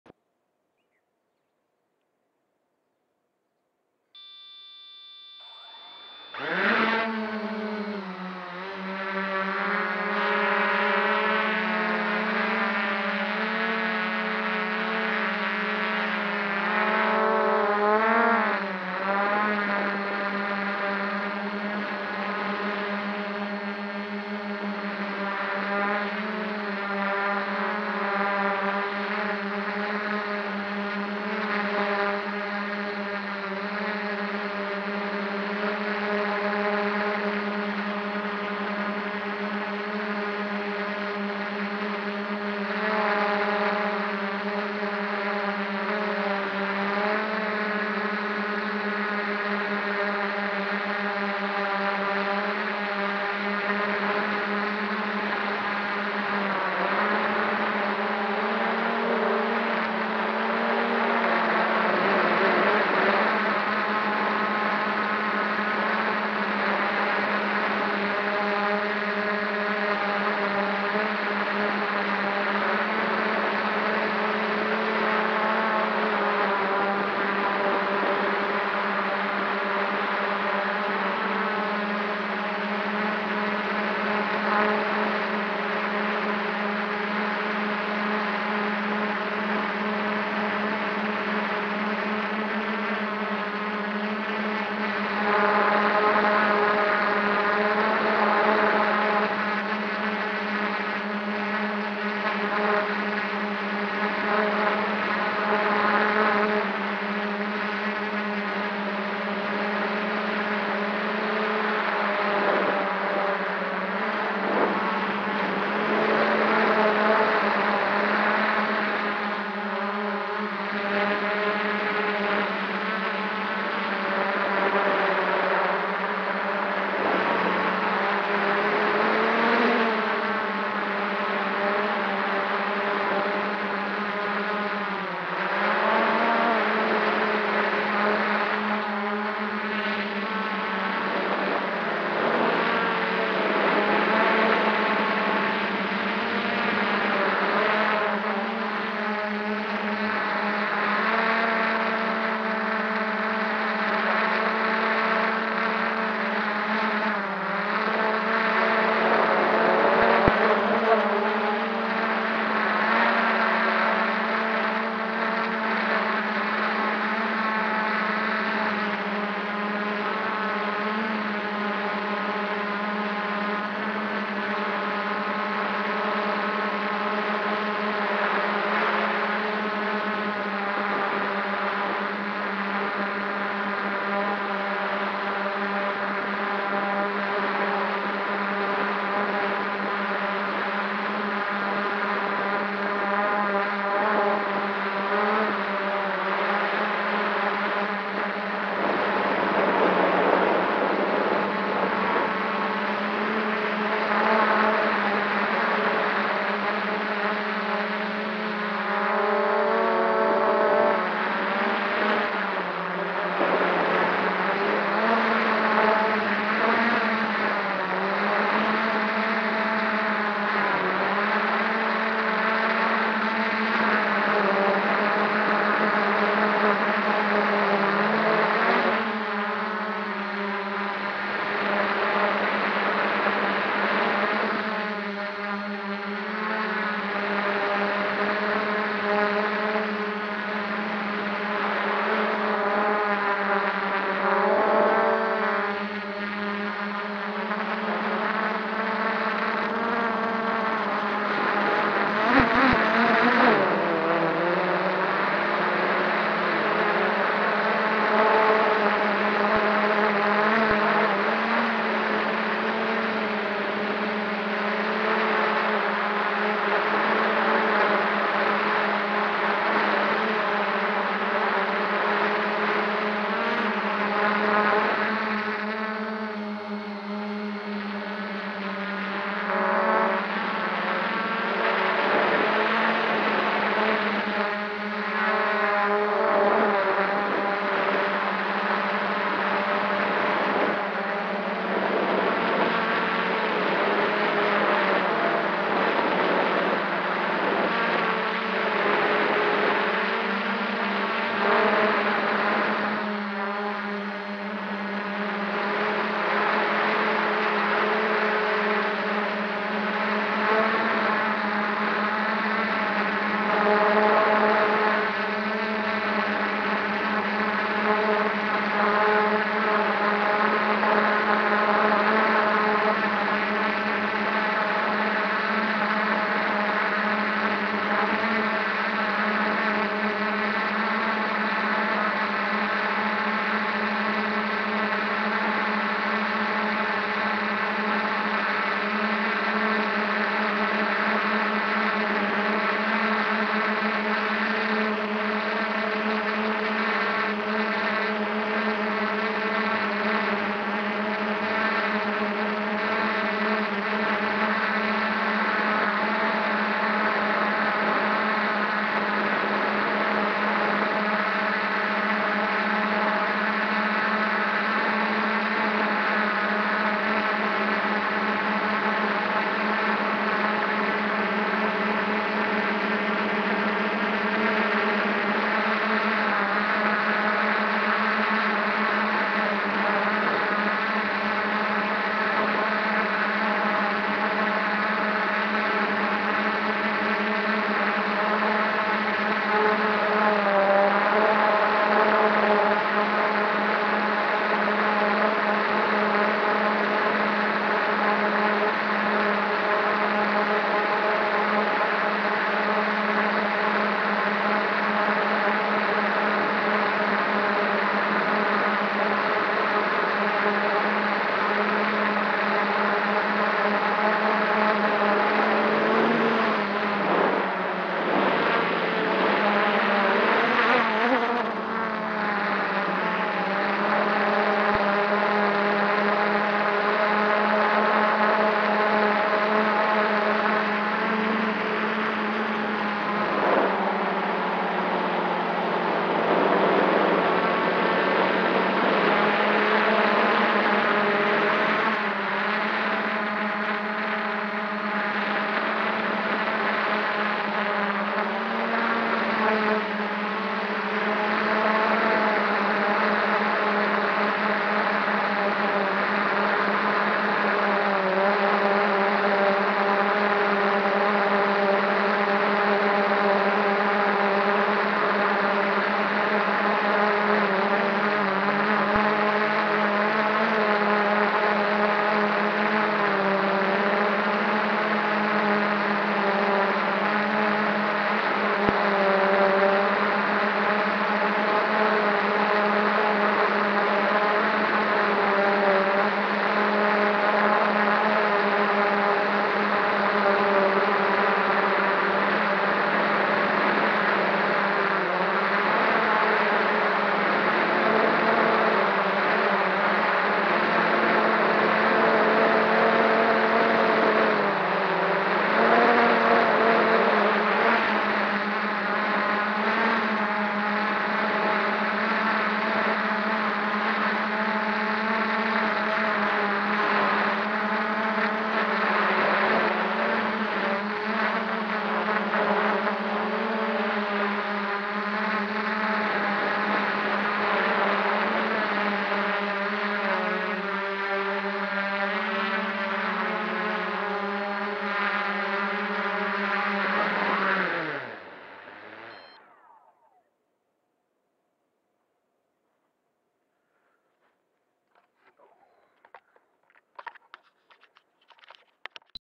ATON1& RUNCAM2
first go with new runcam2 and my ATON Quad May 25 2917